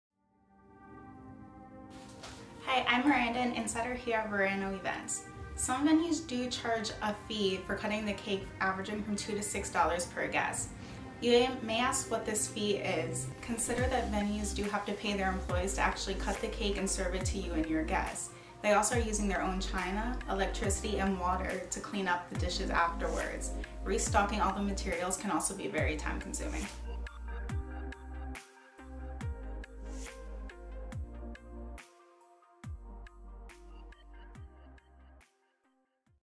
Cake Cutting Fee- Explained
Bringing your own cake to a venue that allows it? They may charge a cake cutting fee.

Paula Anger
Wedding Planner
Delray Beach, FL